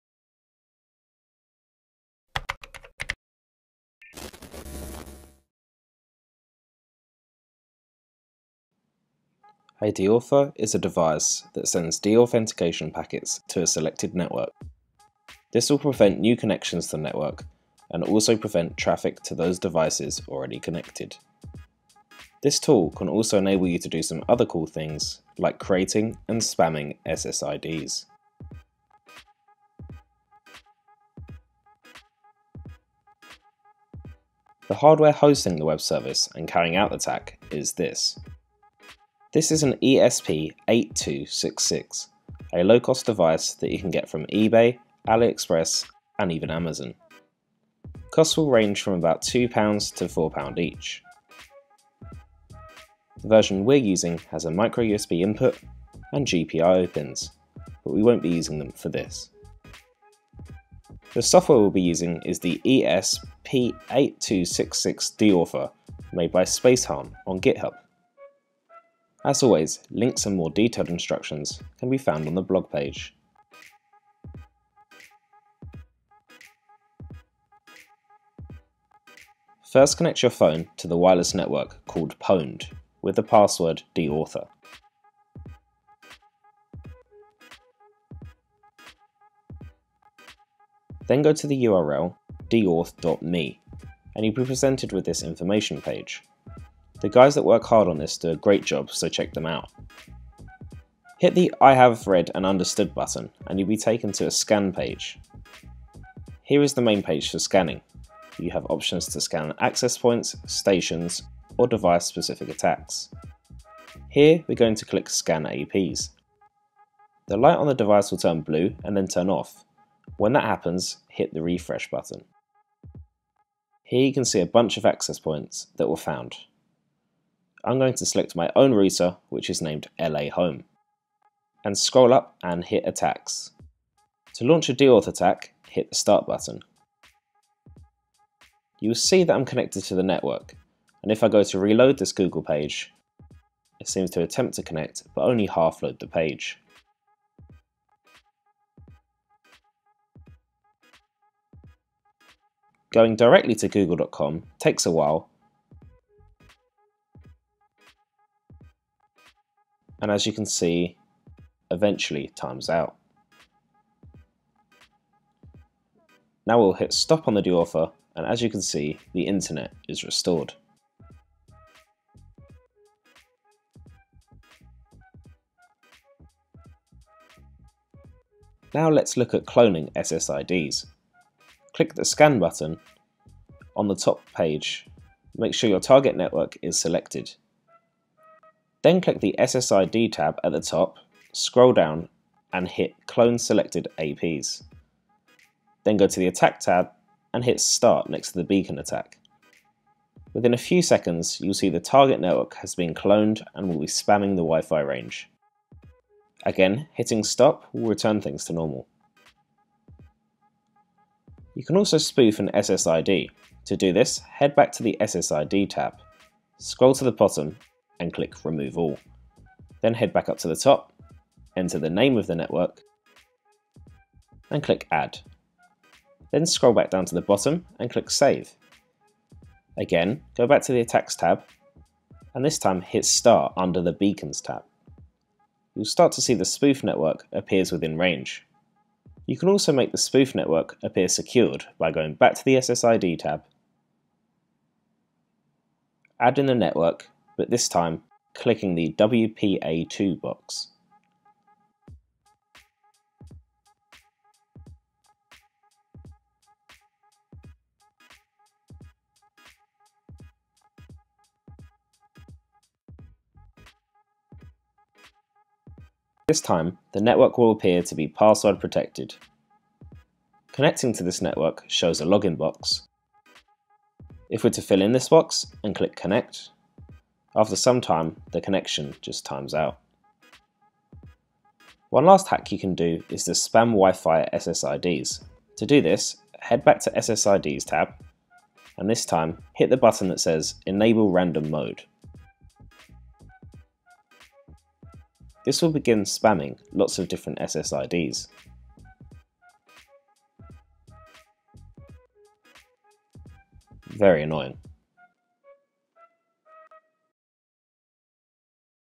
Wifi Deauther
Use an ESP8266 to spam, clone and deauth WIFI.